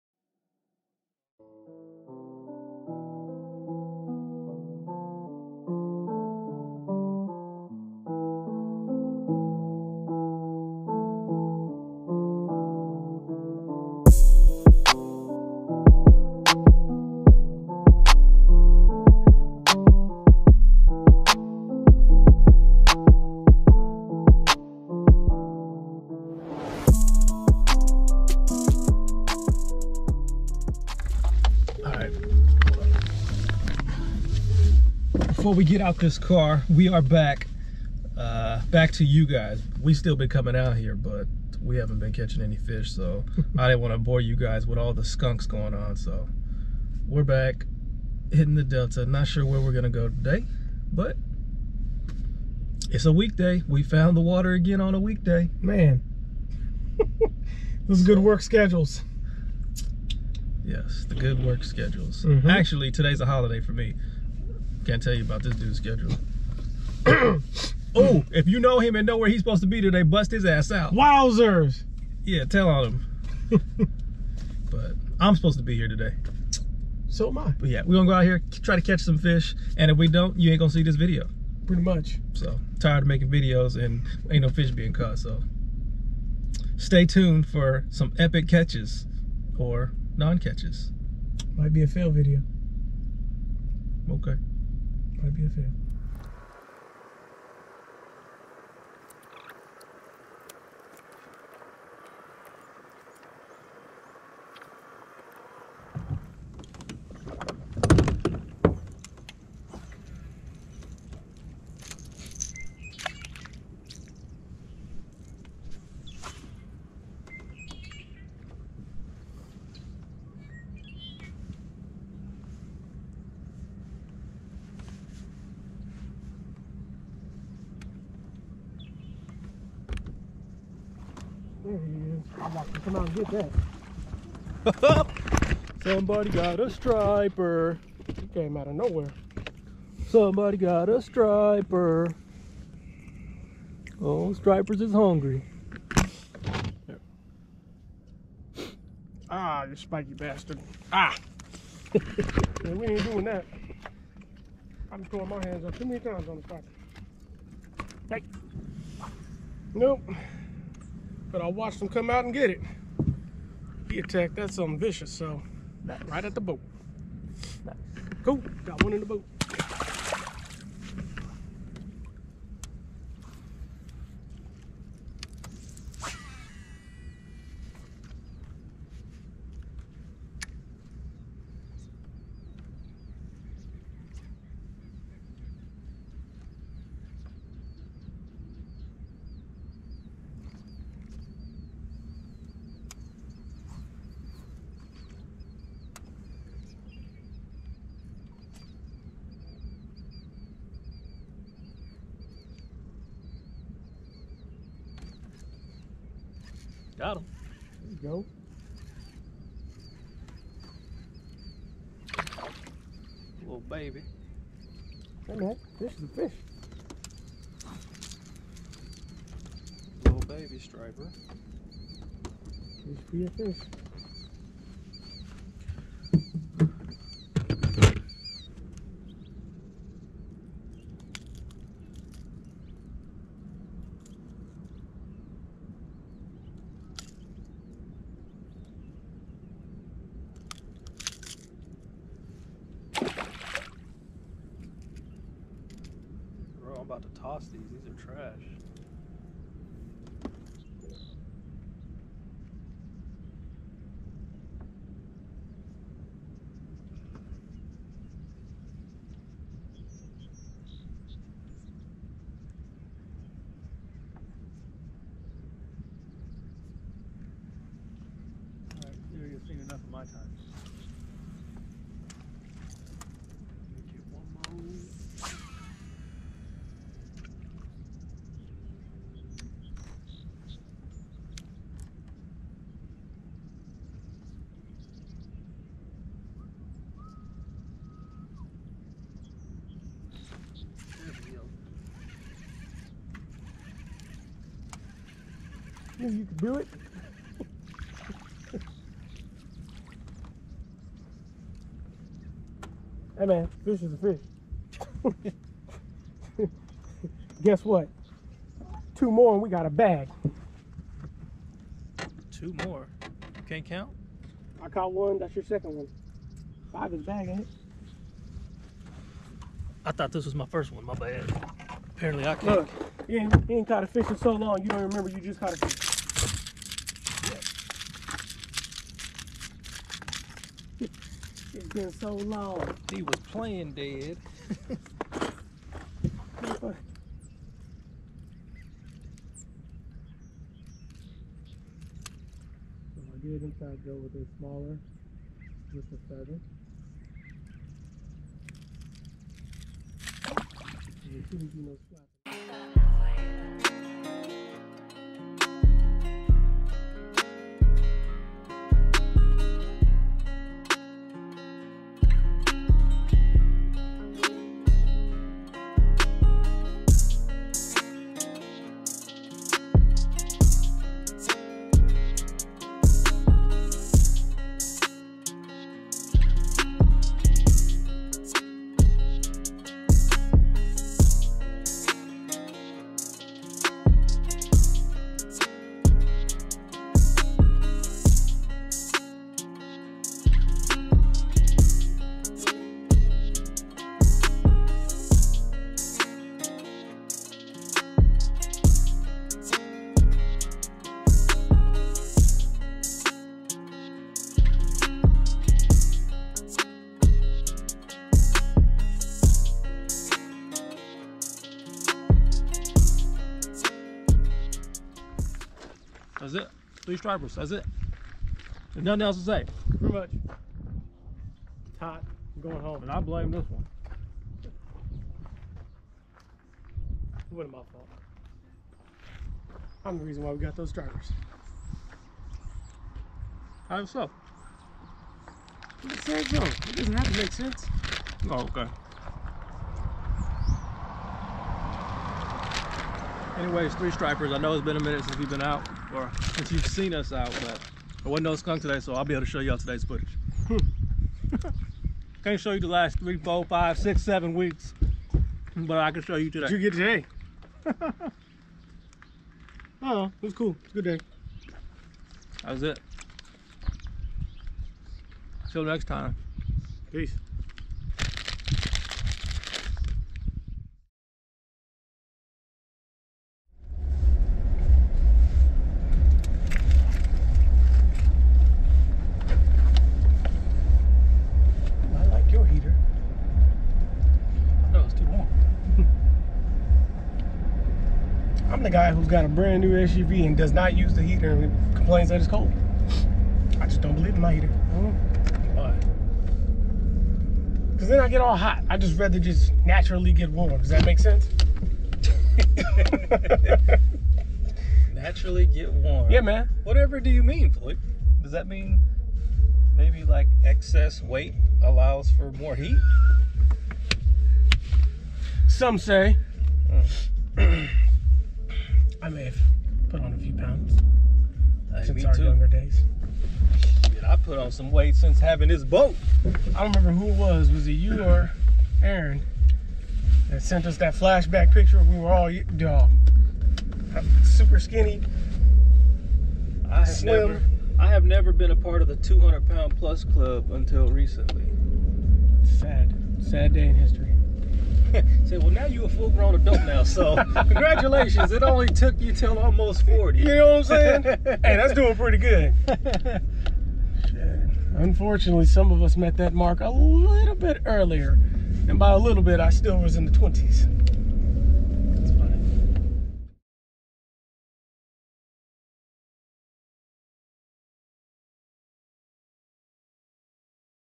Winter fishing at its finest on The California Delta (Yea Right lol)
Well it has been a few weeks since I have posted any videos so be thankful.  The fishing has been more difficult  but we are still at it.  No largemouth today, just a couple of stripers.

Follow Floyd on Instagram @Only1_fr3